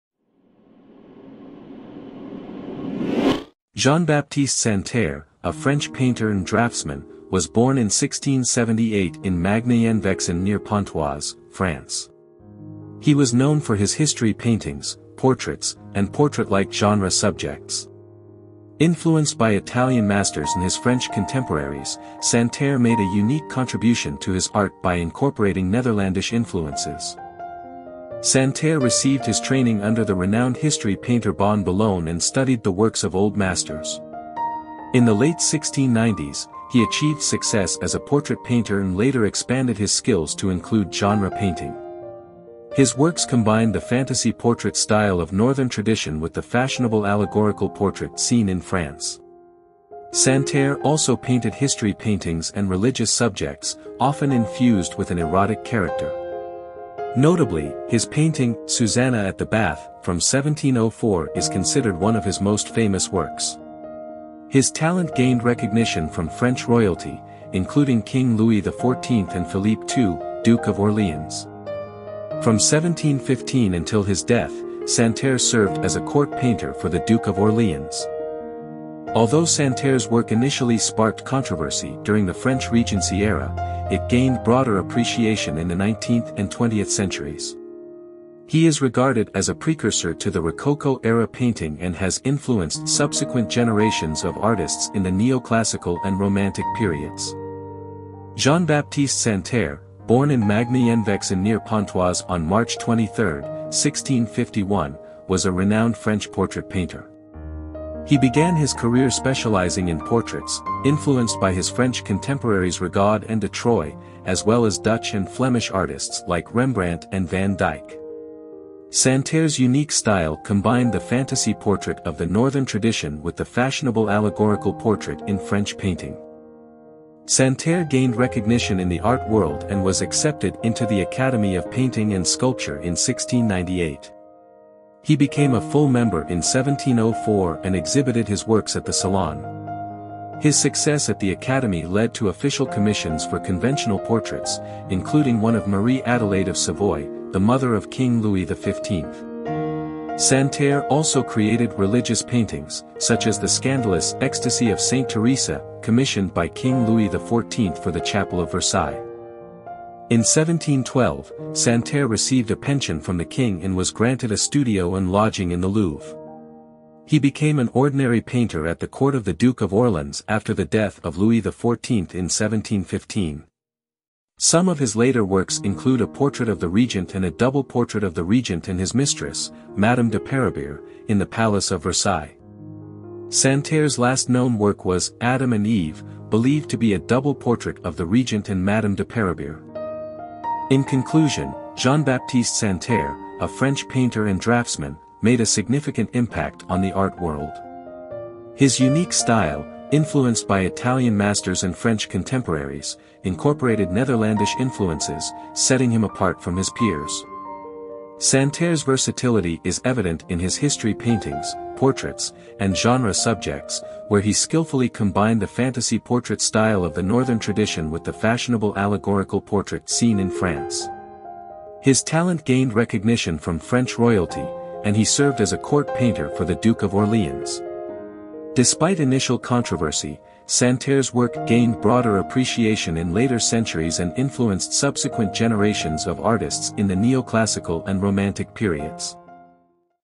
Jean-Baptiste Santerre: Master of French Art｜Artist Biography
Jean-Baptiste Santerre, a French painter and draughtsman, was born in 1678 in Magny-en-Vexin near Pontoise, France

A biography of Jean-Baptiste Santerre.Jean-BaptisteSanterre